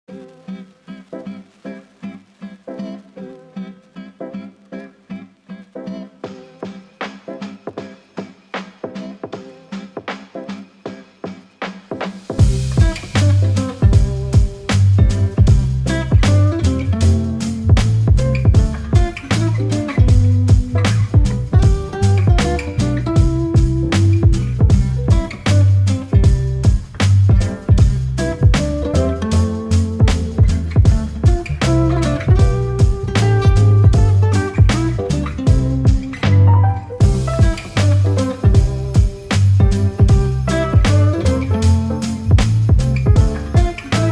Spring Fashion Opener for After Effects 2023
Spring Fashion Opener
First Version – (0:29)
Instagram Version – (0:15)
Project Features
There is No Need to install Plugins
4K ( 3840×2160 ), Full HD ( 1920×1080 )
Easy to Edit
Modular Structure
Multipurpose Project
Well organized
Fast Render